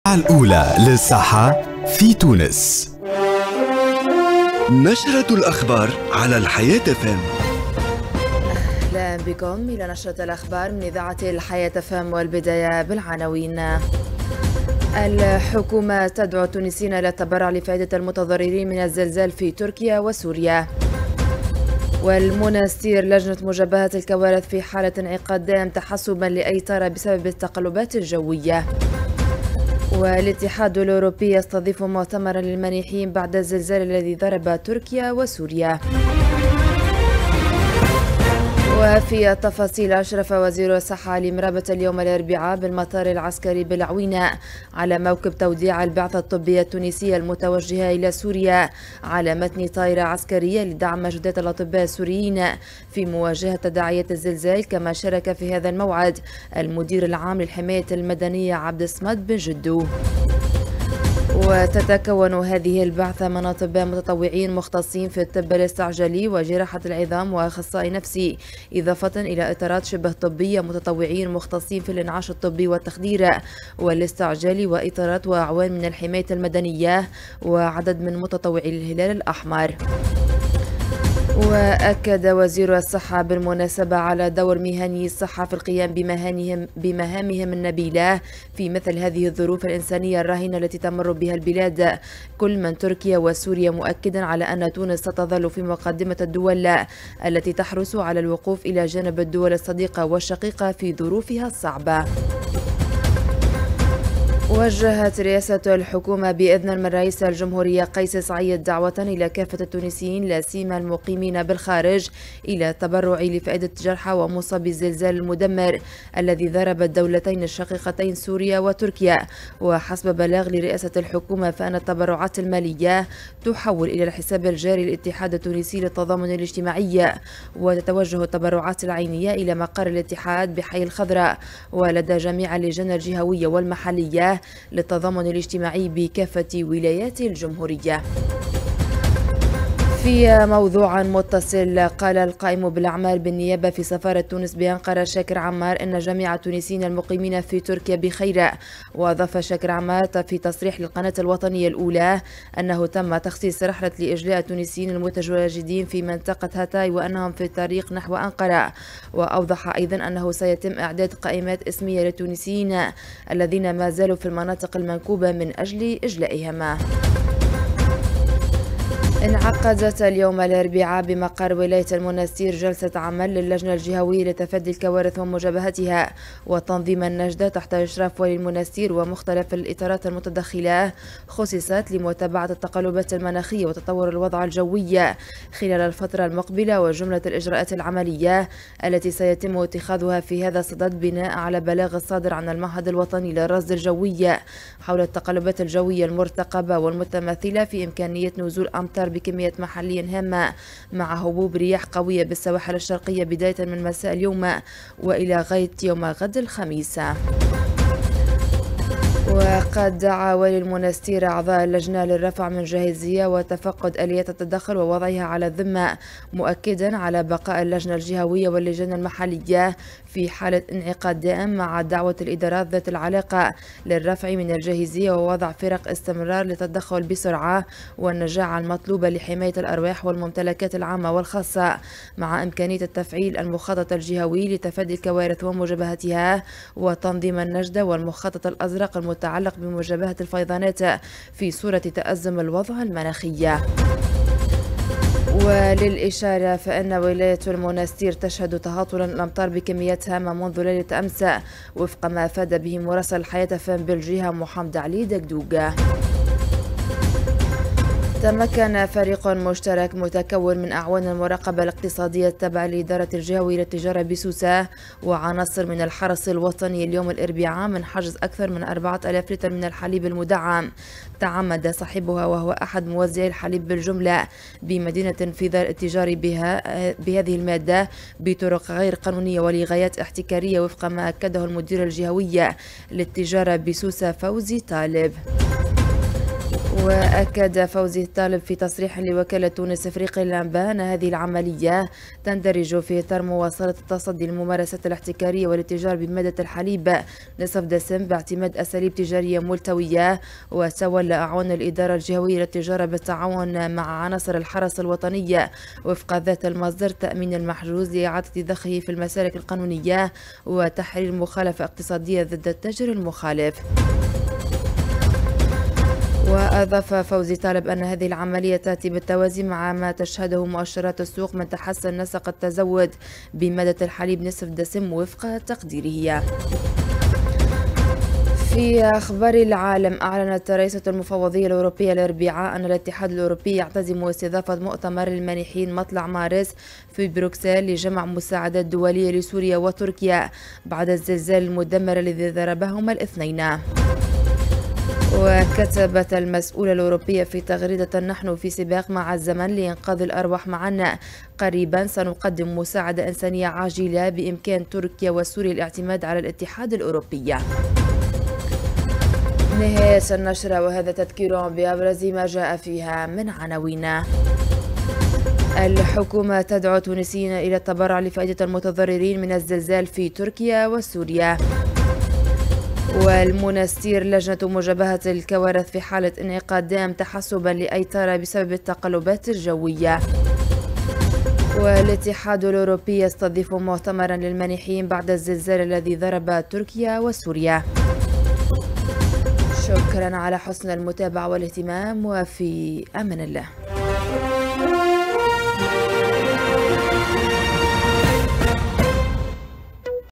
نشرة الاخبار: الحكومة تدعو التونسيين الى التبرع لفائدة المتضررين من الزلزال في تركيا وسوريا
الحكومة تدعو التونسيين الى التبرع لفائدة المتضررين من الزلزال في تركيا وسوريا
المنستير: لجنة مجابهة الكوارث في حالة انعقاد دائم تحسّبا لأي طارئ بسبب التقلبات الجوية
الاتحاد الأوروبي يستضيف مؤتمرا للمانحين بعد الزلزال الذي ضرب تركيا وسوريا
البث المباشر🎙📻🎚